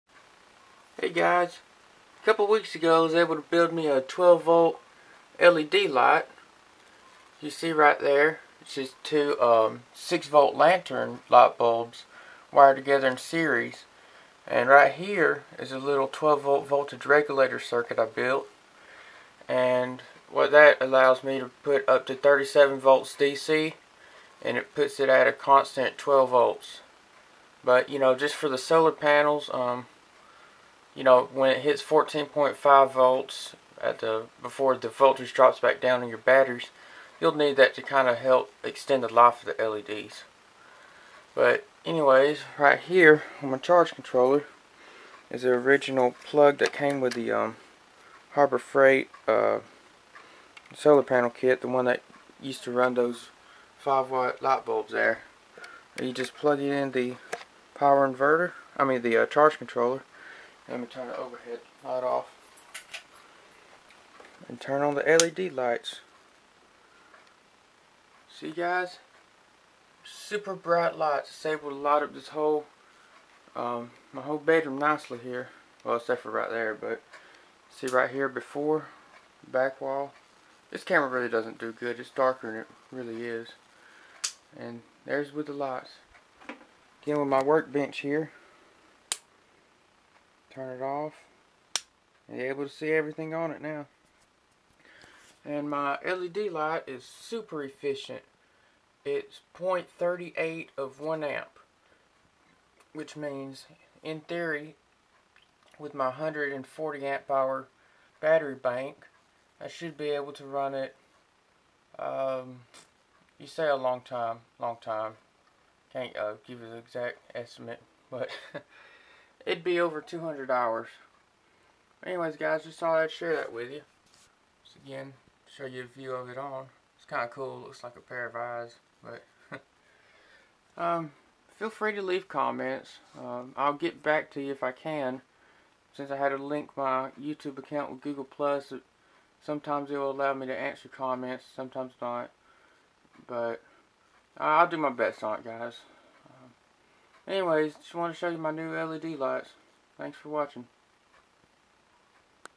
Homemade 12Volt L.E.D. Lights
I made these lights out of two 6volt ten L.E.D. lanterns. The light only uses 0.38 amps (that's super efficient). The light is 170 lumens.